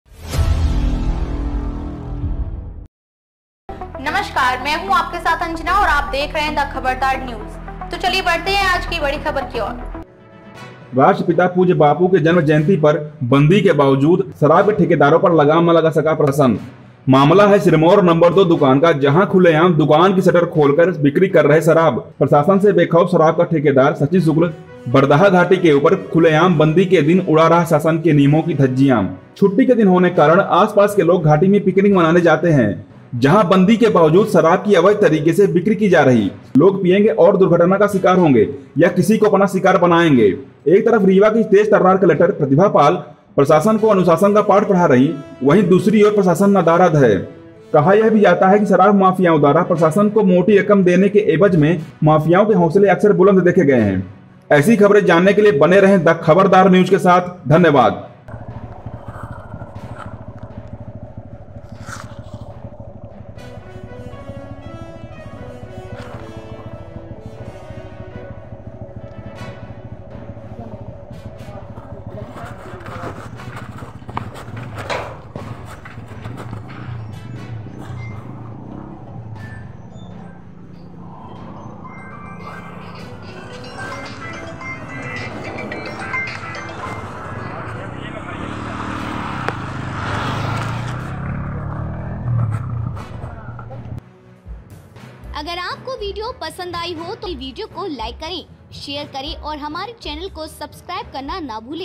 REWA NEWS - गांधी जयंती के दिन रीवा में खुले आम बिक रही शराब | The Khabardar News
राष्ट्रपिता*पूज्य*बापू*के जन्म*जयंती*पर*बंदी*के बावजूद*शराब*के*ठेकेदारों*पर लगाम*न*लगा*सका प्रशासन
मामला है सिरमौर नंबर-2 दुकान का जहां खुले आम दुकान की शटर खोलकर बिक्री कर रहे शराब,प्रशासन से बेख़ौफ़ शराब का ठेकेदार सचिन शुक्ल बरदहा घाटी के ऊपर खुलेआम बंदी के दिन उड़ा रहा शासन के नियमों की धज्जियाँ, छुट्टी का दिन होने के कारण आस-पास के लोग घाटी में पिकनिक मनाने जाते हैं जहां बंदी के बावजूद शराब की अवैध तरीके से बिक्री की जा रही लोग पियेगें और दुर्घटना का शिकार होंगे या किसीको अपना शिकार बनाएंगे,एक तरफ रीवा की तेज तर्रार कलेक्टर प्रतिभा पाल प्रशासन को अनुशासन का पाठ पढ़ा रहीं वही दूसरी ओर प्रशासन नदारद है,कहा यह भी जाता है कि शराब माफियाओं द्वारा प्रशासन को मोटी रकम देने के एवज में माफियाओं के हौसले अक्सर बुलंद देखे गयें हैं।